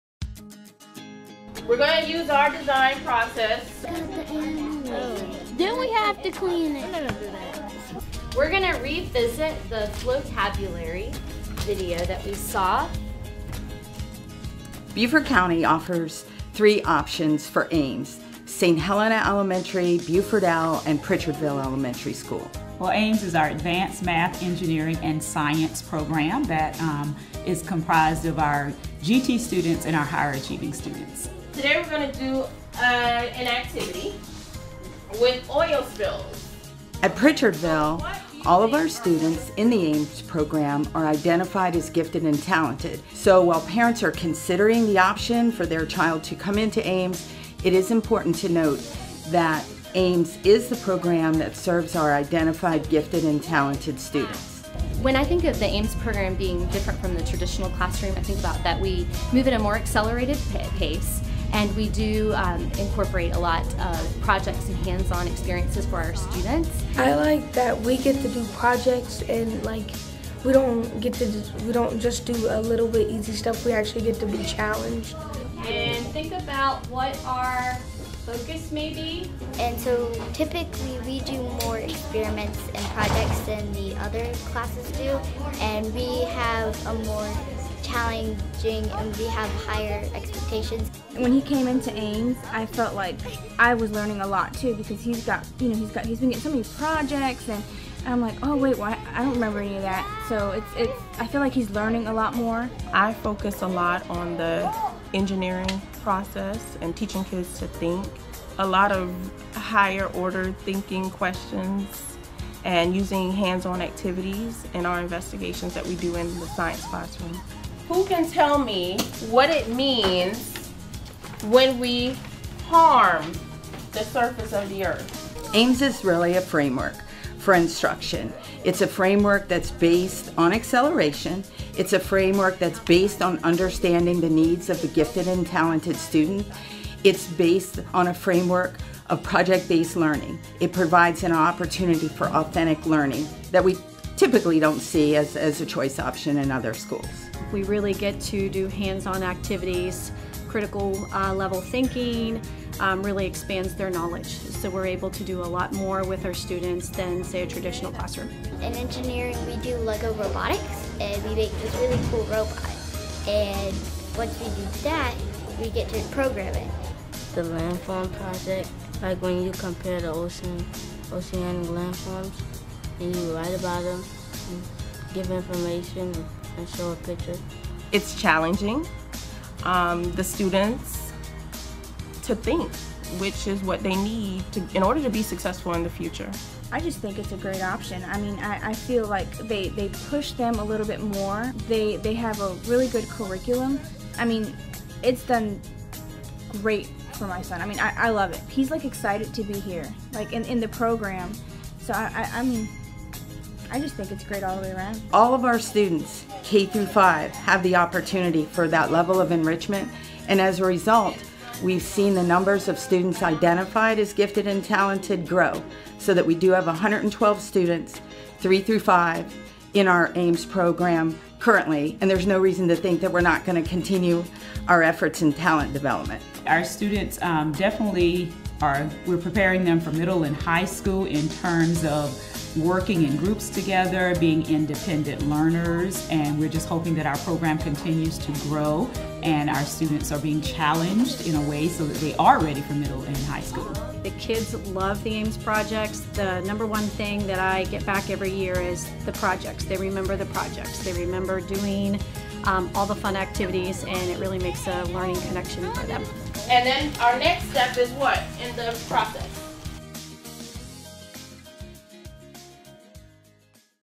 AMES Program
The Beaufort County School District offers several school choice options for parents and students, here’s a look at the Advanced Math Engineering & Science Academy (AMES) Program being offered at Beaufort Elementary, Pritchardville Elementary and St. Helena Elementary Schools.